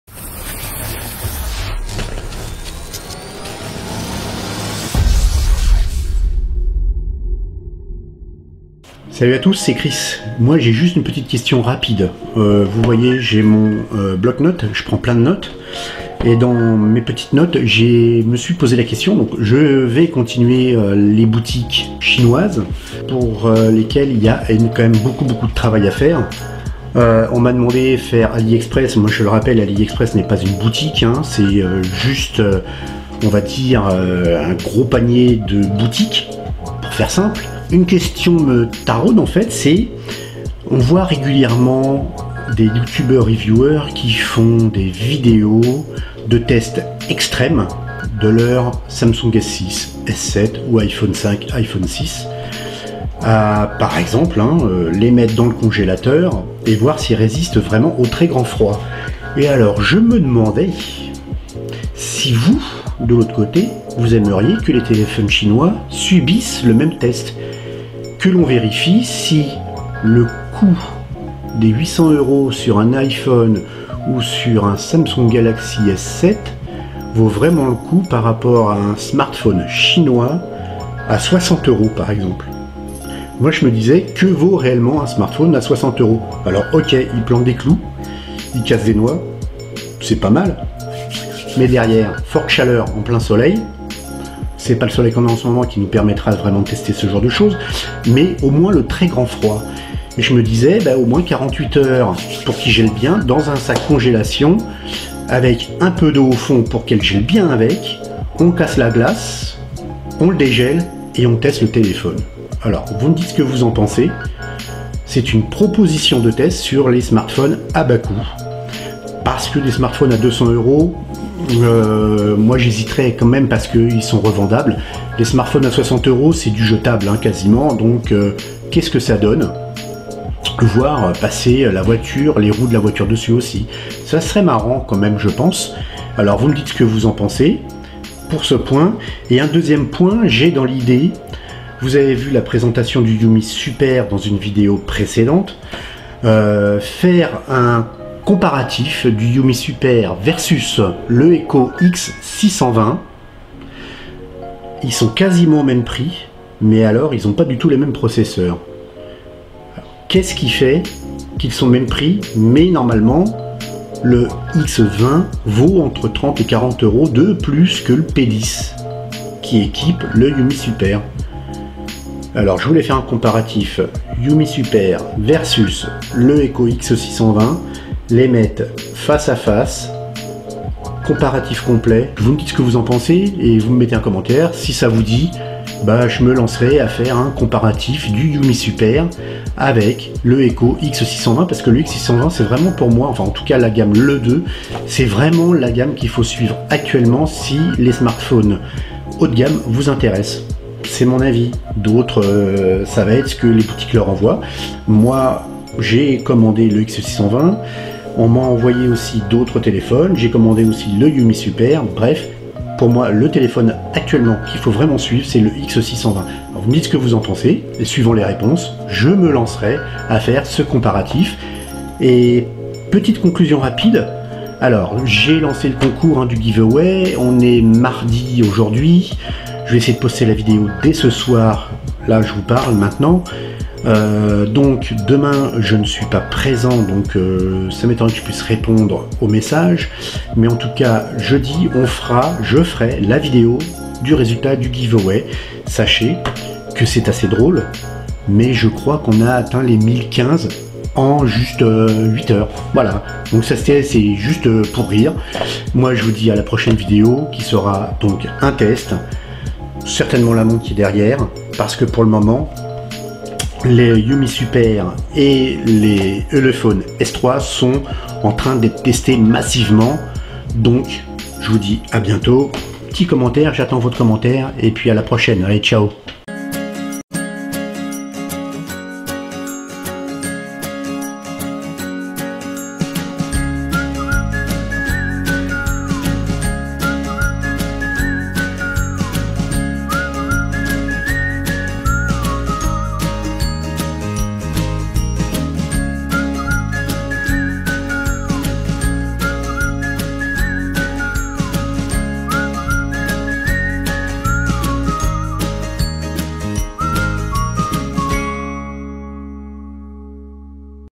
VIDEO Participative : QUESTIONS et PROPOSITIONS pas malhonnêtes - SinoDroidTechVlog
VLOG QUESTIONS/PROPOSITIONS

Youtube est, avant tout, un réseau social et beaucoup on oublié son intention première.
C'est donc pour cette raison que je fais cette vidéo participative afin de faire évoluer cette chaîne suivant vos goûts et propositions !
Alors n'hésitez pas à nous laisser un commentaire, constructif je l'espère, nous vous écouterons et ferons le maximum pour répondre et réaliser vos demandes :) bien entendu dans la limite du raisonnable ^^

Bref, à vous de jouer maintenant et faites vos propositions !